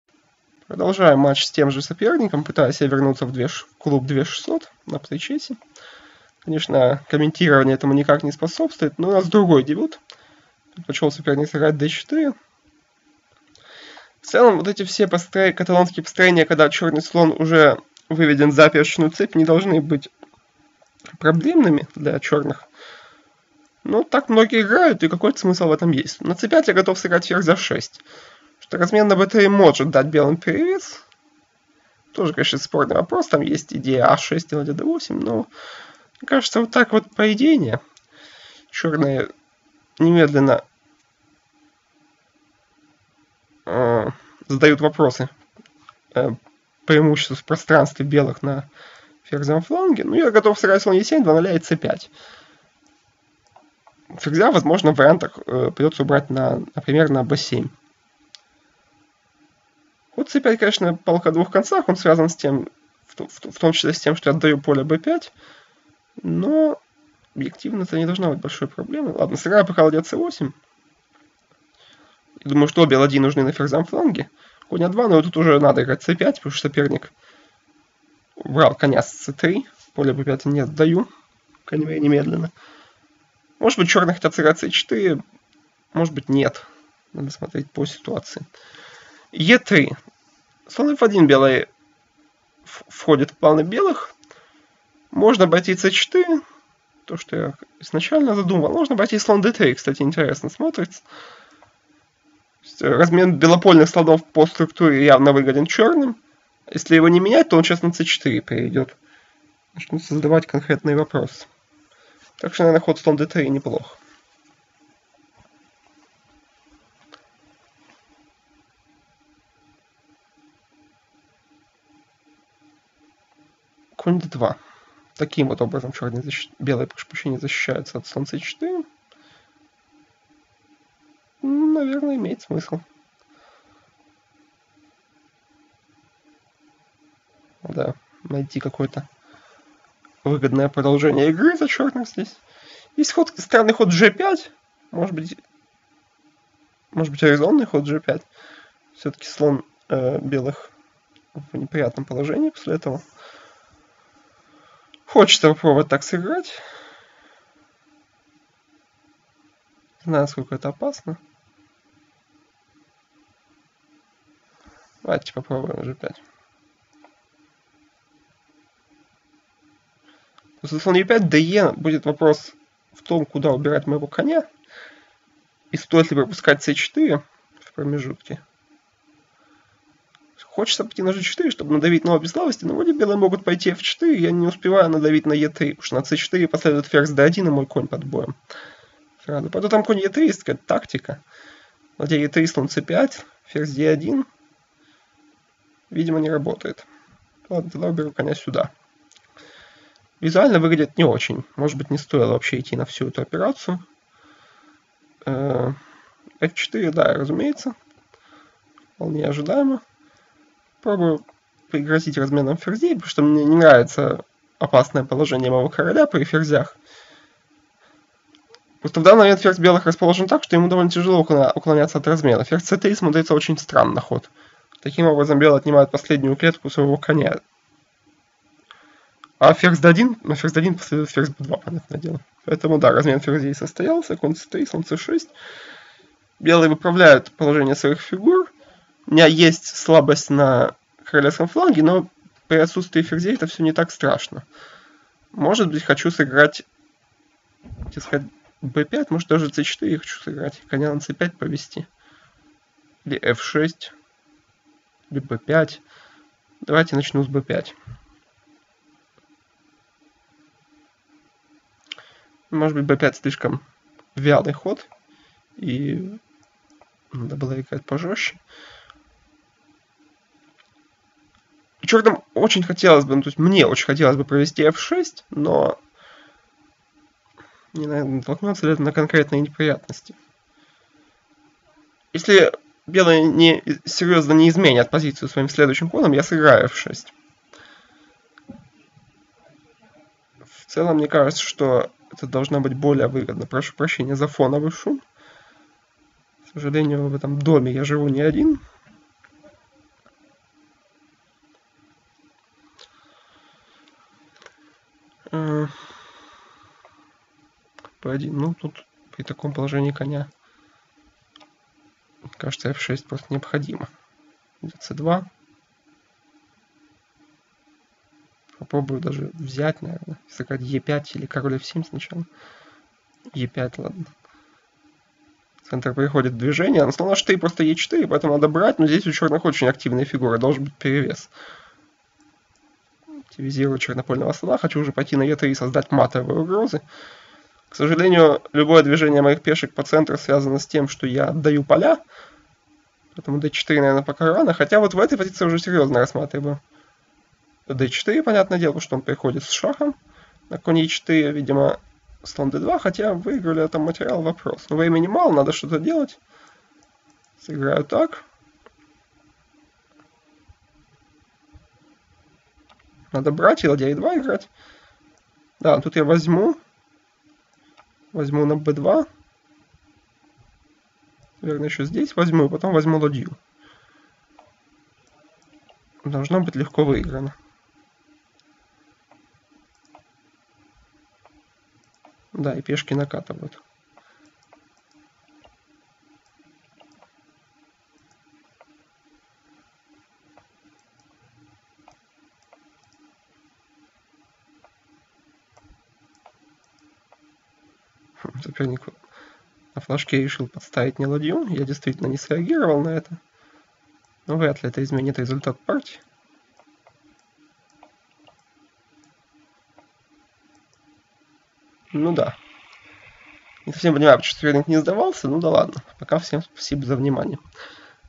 Ночной блиц №13. Славянская защита. Вариант 3. g3?!
Есть желание поблагодарить авторов и поддержать творческую активность?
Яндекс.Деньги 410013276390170